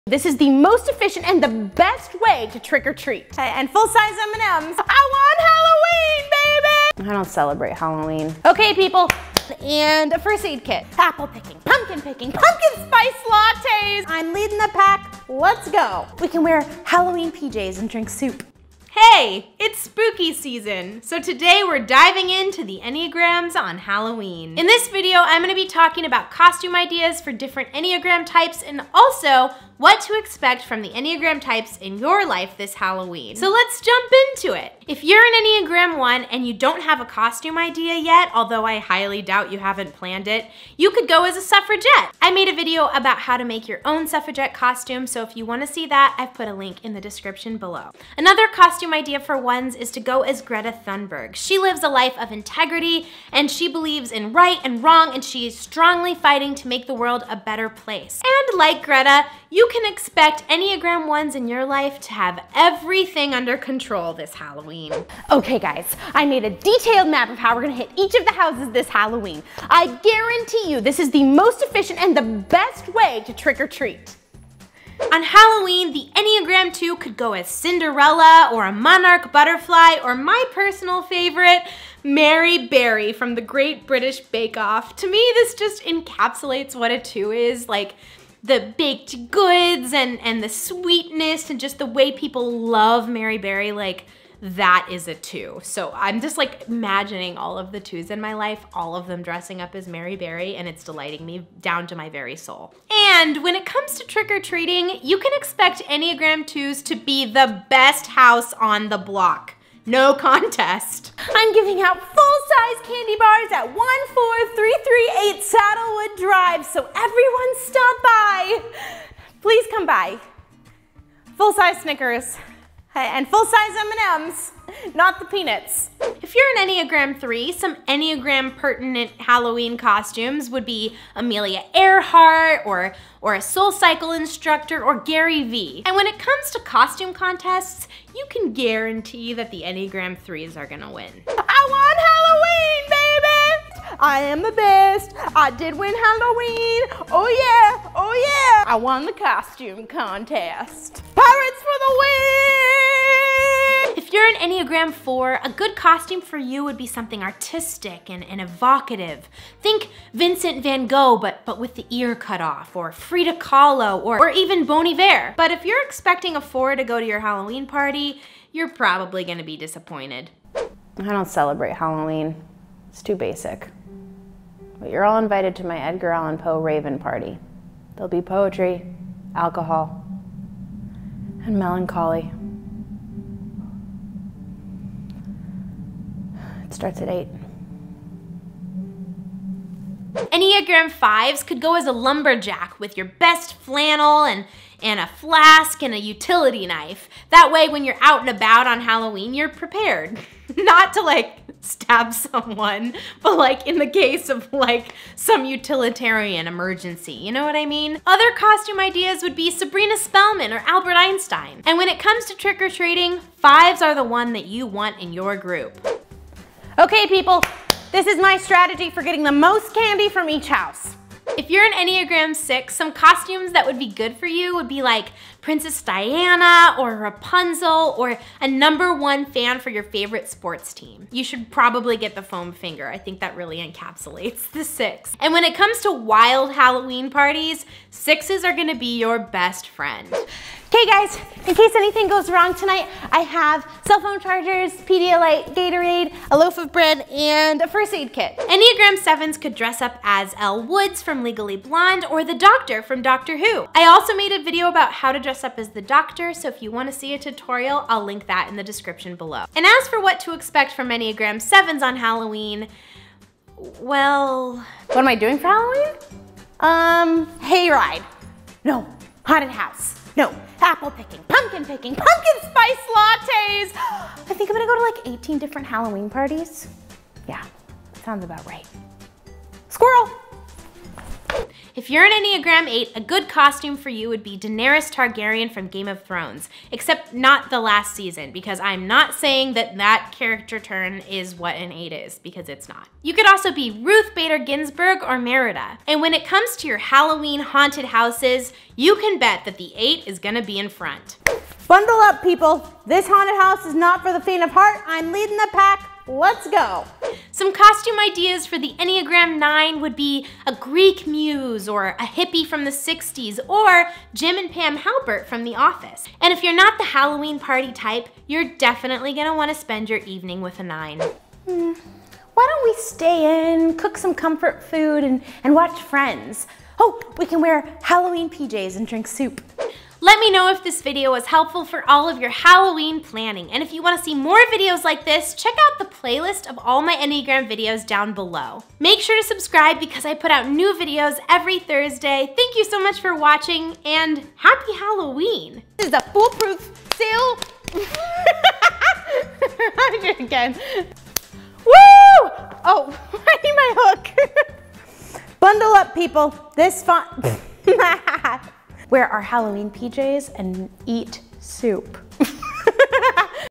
Enneagram Types on Halloween 🎃
Find out what costume is the best fit for your Enneagram type on Halloween! Also learn what to expect from the Enneagram types in your life this October, especially when it comes to trick or treating.